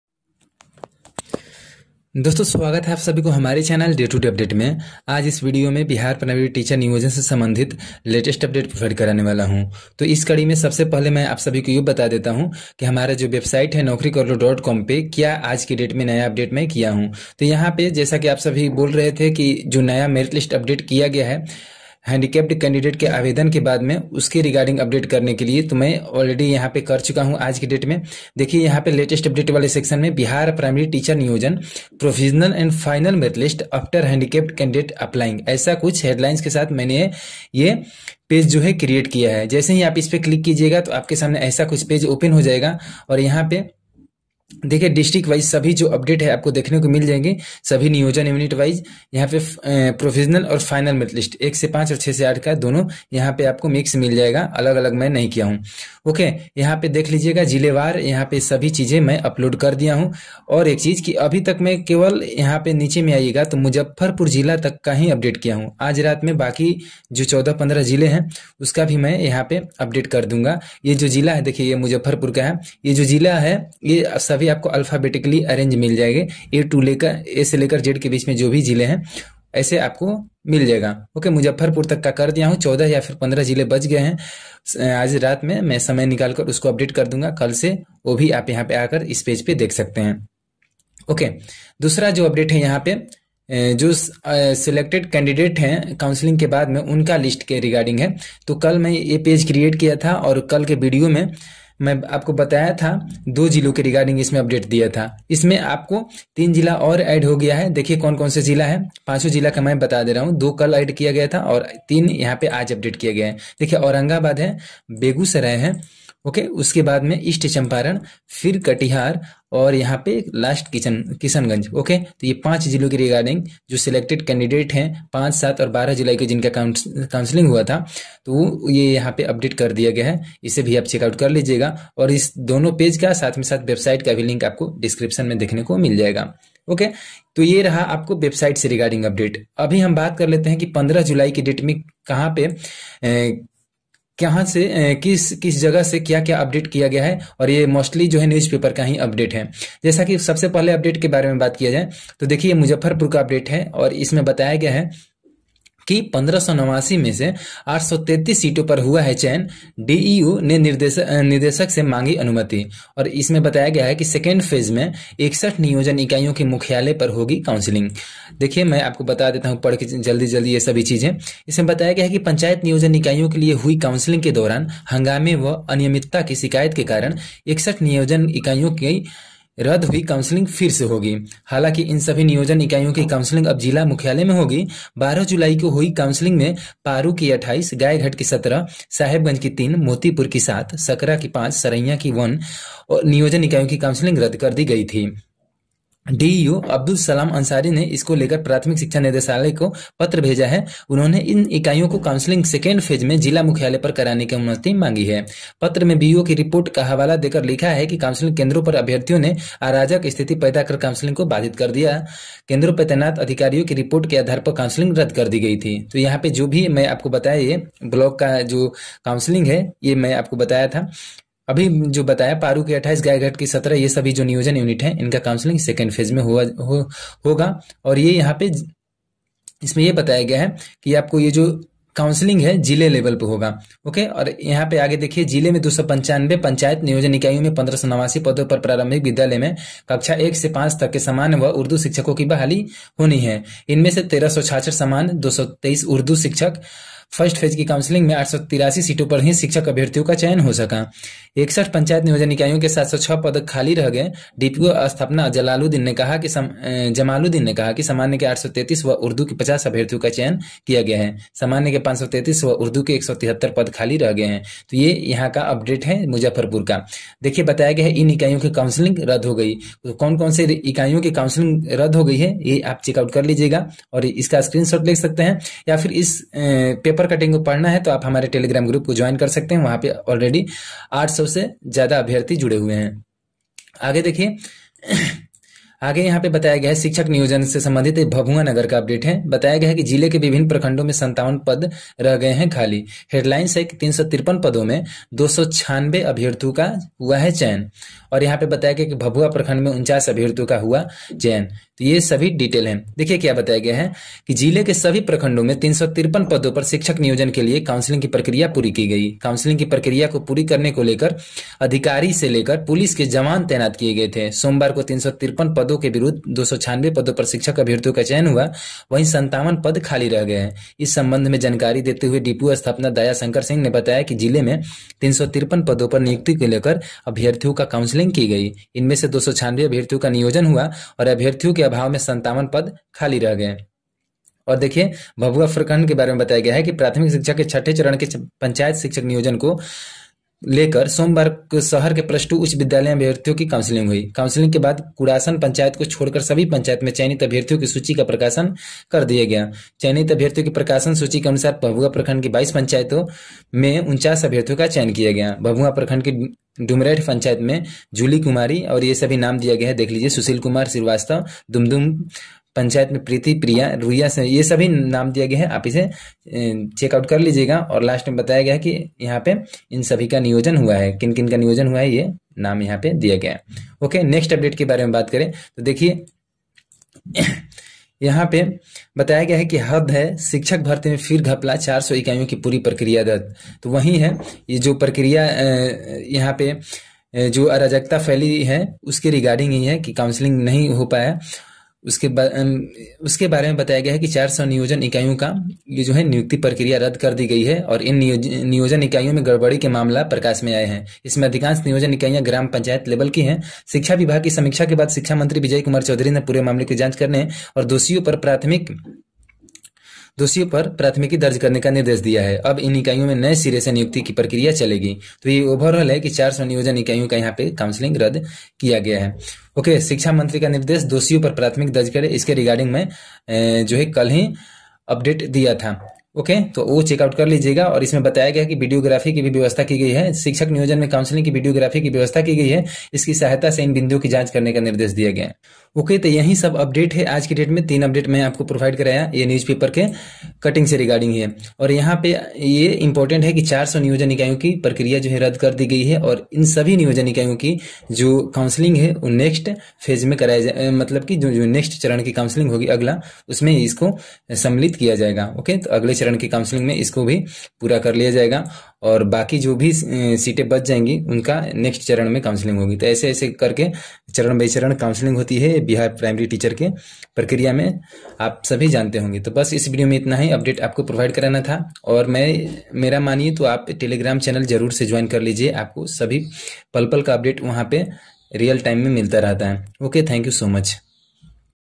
Bihar Primary Teacher Latest Updates of 15th July 2021 | New Merit List | Selected candidates List
Bihar Primary Teacher Latest Updates of 15th July 2021

Bihar Primary Teacher Counselling Updates

Bihar Primary Teacher Provisional and Final Merit List After handicapped candidates Applying